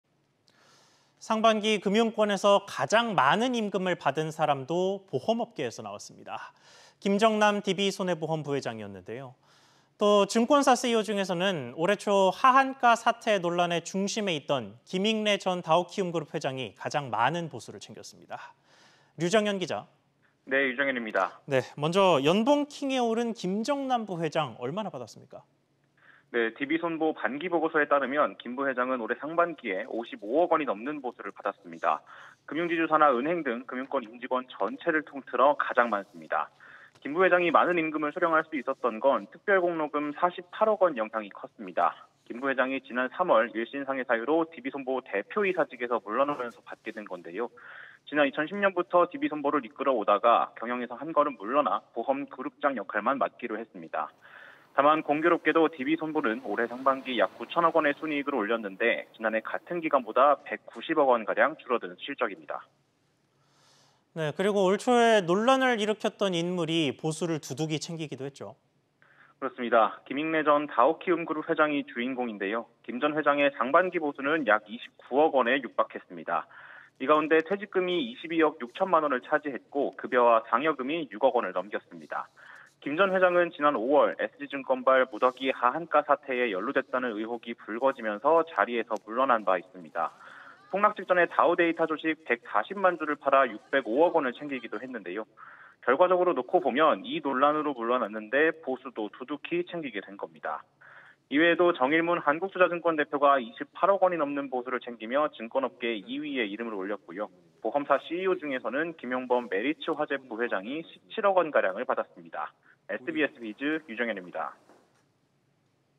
'금융권 연봉킹' DB손보 김정남…'하한가 사태 논란' 김익래, 증권업계 1위
[앵커]

상반기 금융권에서 가장 많은 임금을 받은 사람도 보험업계에서 나왔습니다.

김정남 DB손해보험 부회장이었는데요.

증권사 CEO 중에서는 올 초 하한가 사태 논란의 중심에 있던 김익래 전 다우키움그룹 회장이 가장 많은 보수를 챙겼습니다.

류정현 기자, 먼저 연봉킹에 오른 김정남 부회장은 얼마나 받았습니까?